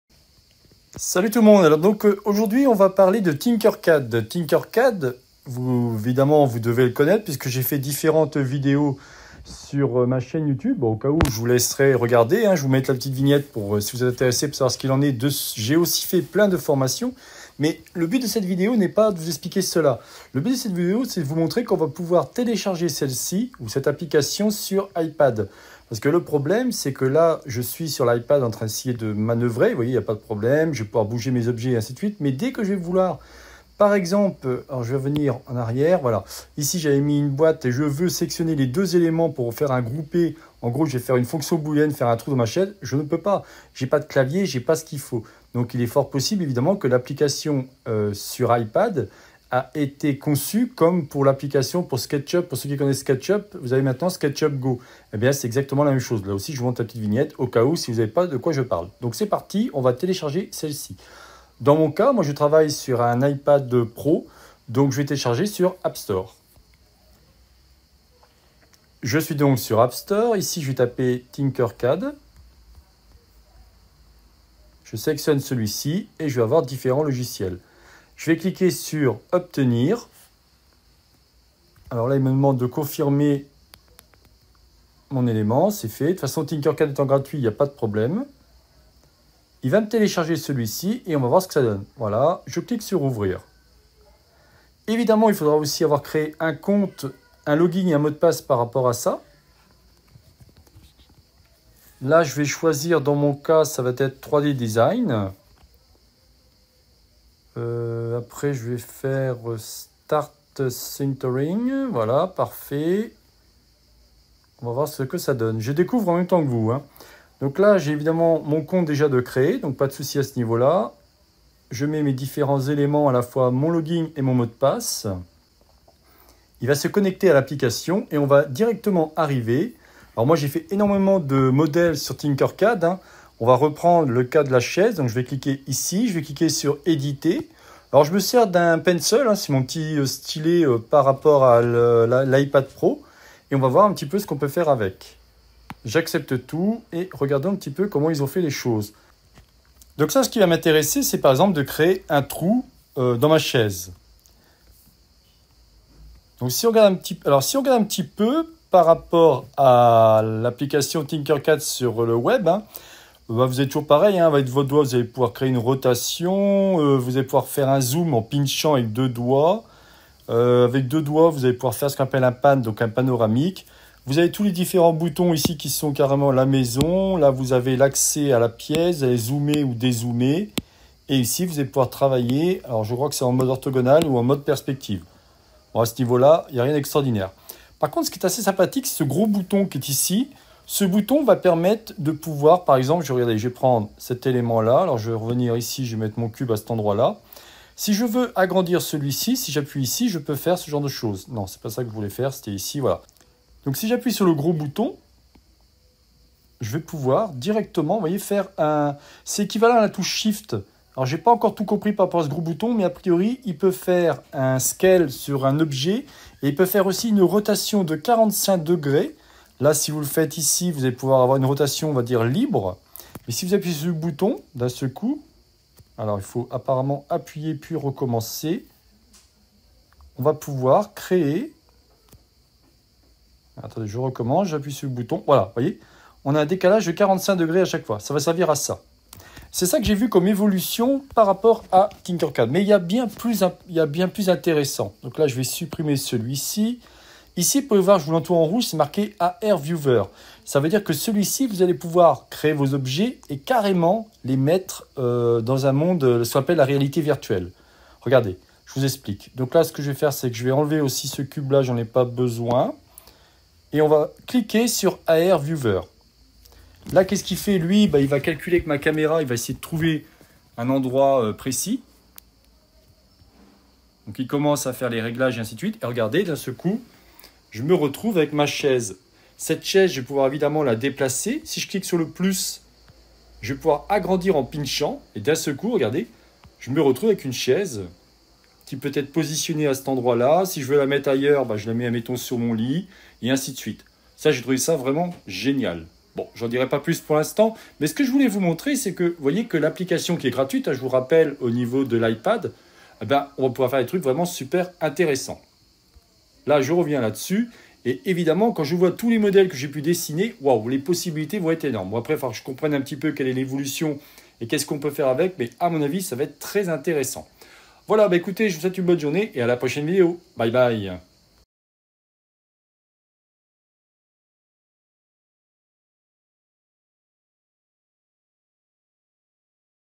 TinkerCAD - 01 - for Ipad / pour un Ipad
Comme je l'ai fait sur Sketchup Go, je vais vous présenter l'installation de Tinkercad mais celui-ci réserve une super surprise, c'est la réalité augmenté, on va pouvoir placer nos objets directement dans un endroit mais cette fois-ci, puis ajusté et modifié le modèle.